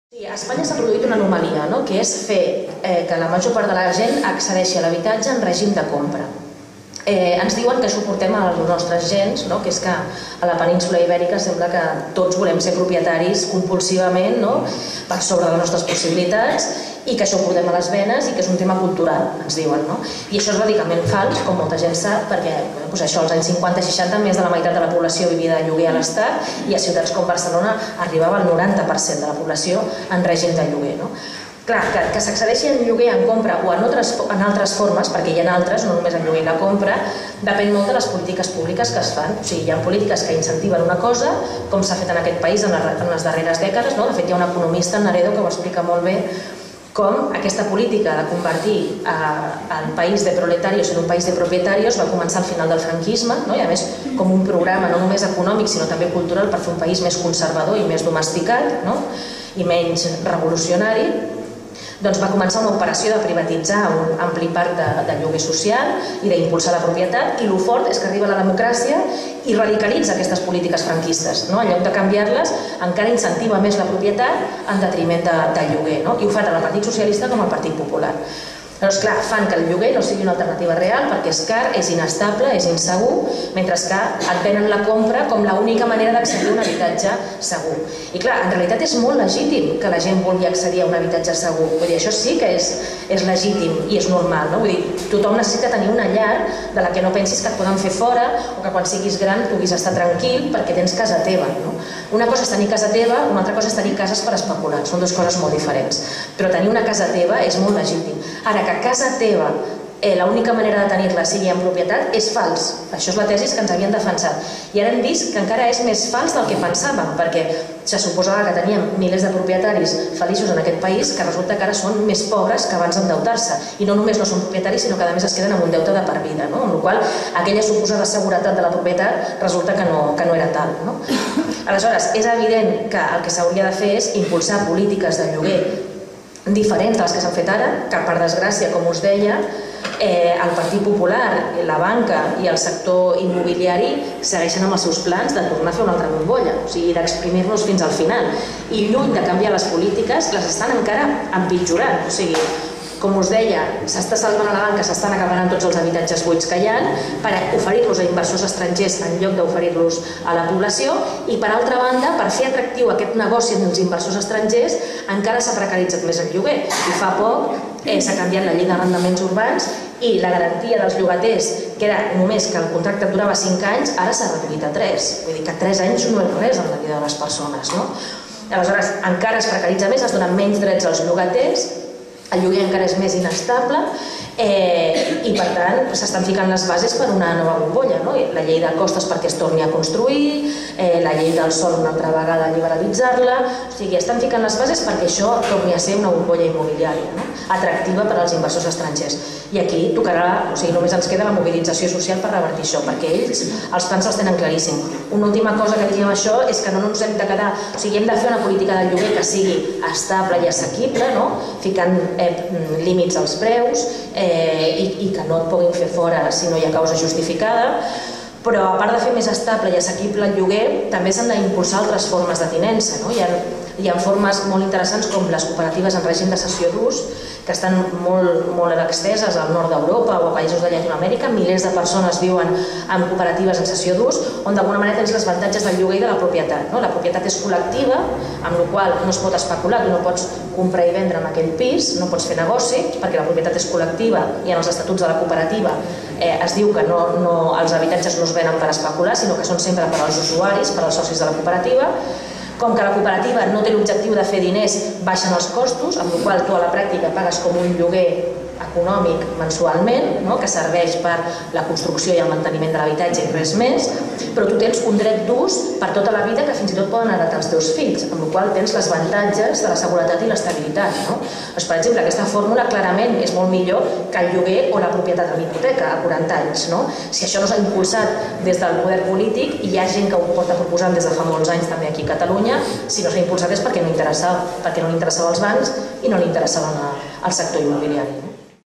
Ada Colau - Cultura de la compra d'habitatge (9 de 18)
Ada Colau participa a l'espai "Cafè amb..." organitzat per ICV L'Hospitalet.
Ada Colau parla sobre la cultura de la compra d'habitatge a Catalunya i Espanya i les polítiques darrera aquesta cultura.